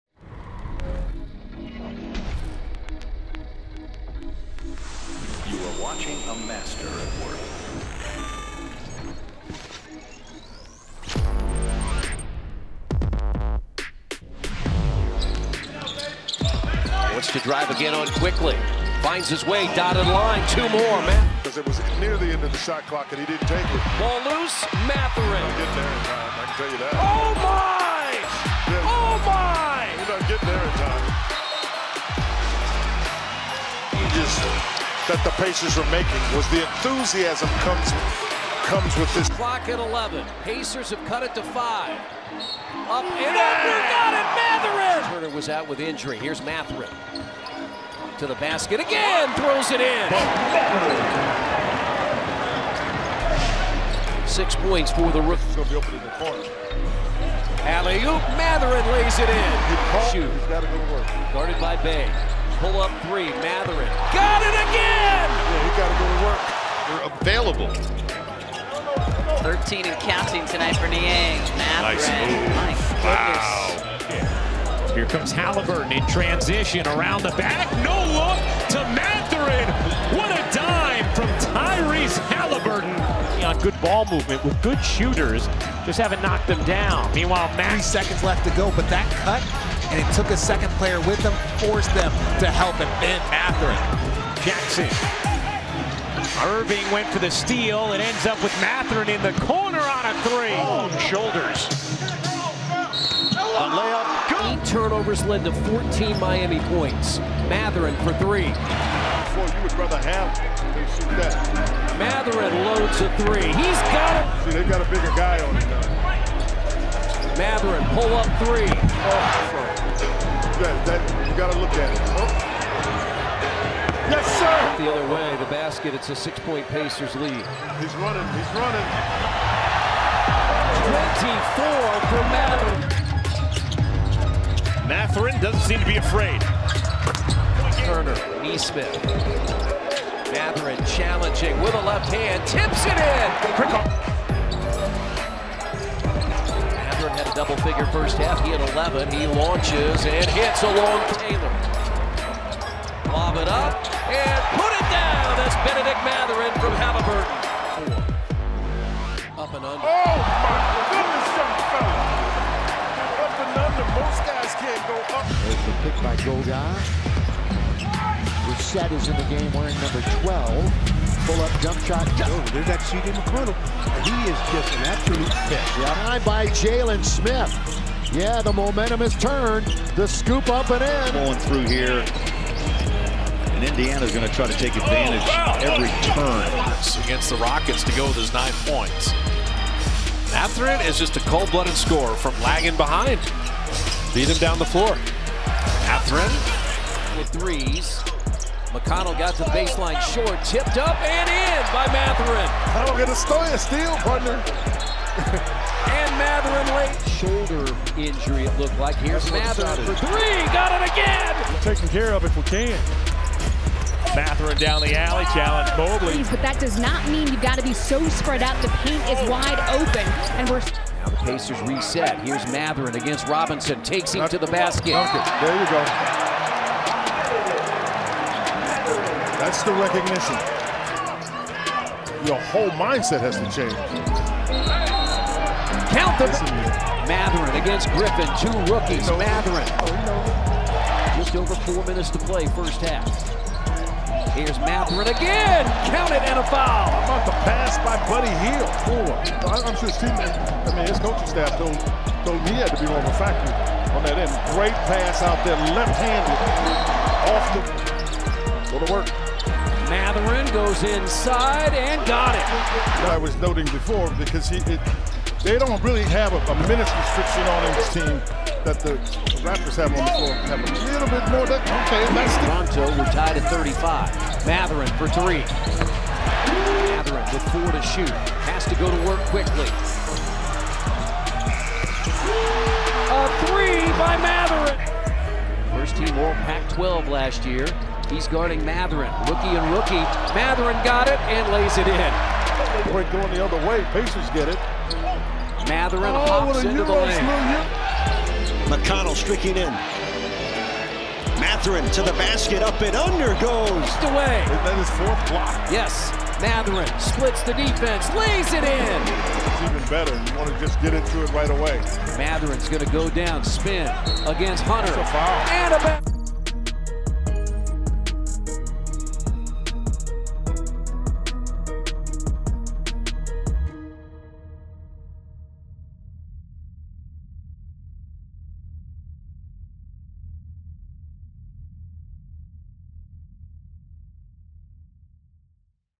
Bennedict Mathurin Is COLD BLOODED! 🏁🔥
Bennedict Mathurin best moments & highlights from the 22-23 NBA Season! as of Jan 20 2023